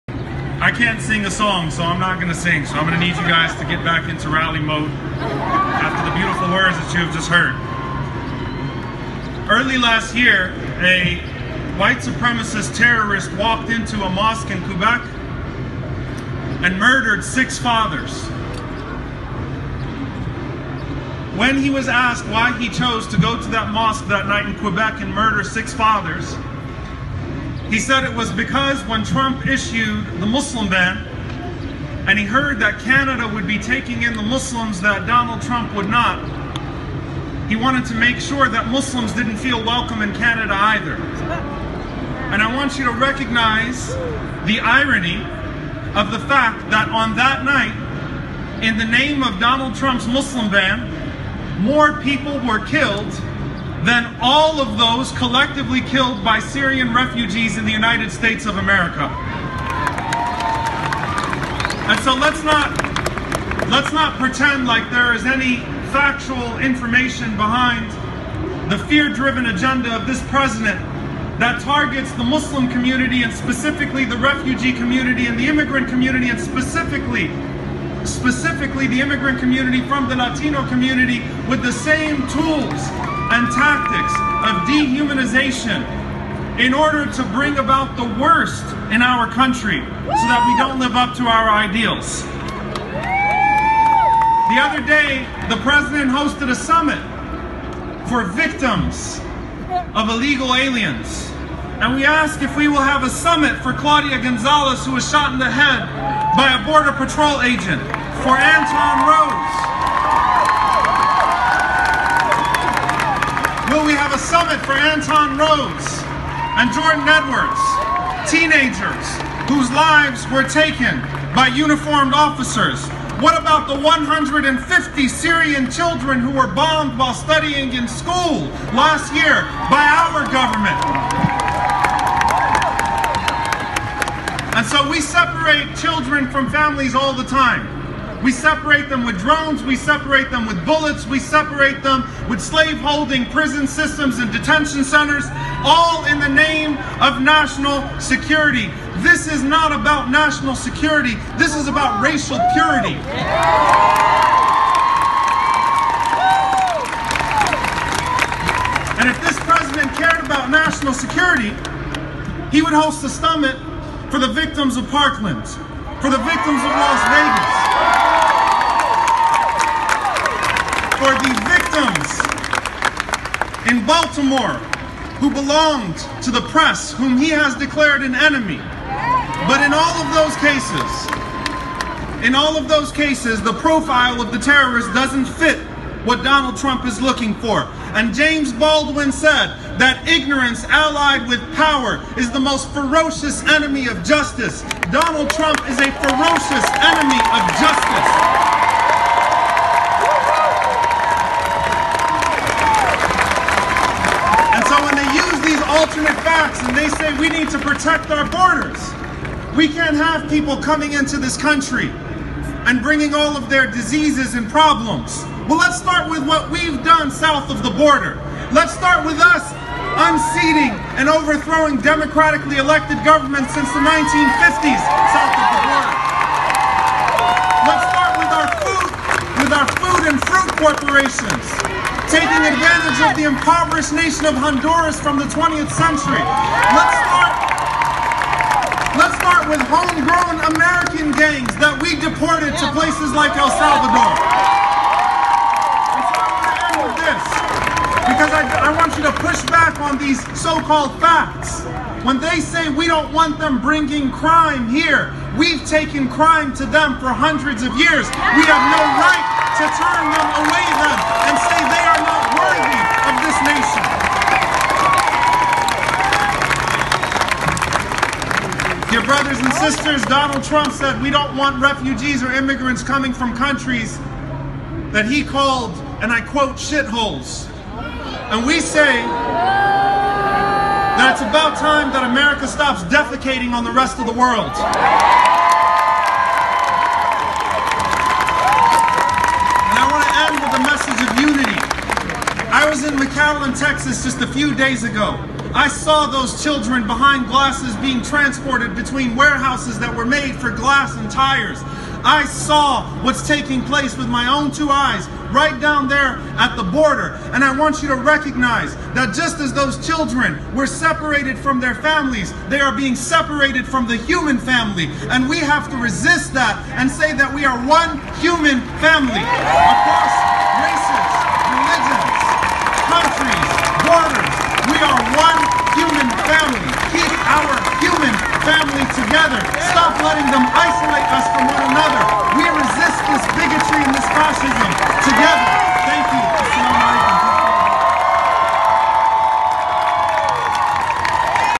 Omar Suleiman speaks at Families Together March and Rally | CAIR-LA
On Saturday, June 30, CAIR-LA cosponsored the "Families Belong Together" rapid response march in LA. Imam Omar Suleiman spoke about the intersections between the Muslim Ban's agenda to divide families and the tactics used to break families at the border. We stand in solidarity with the immigrants and refugees suffering from this administration's policies.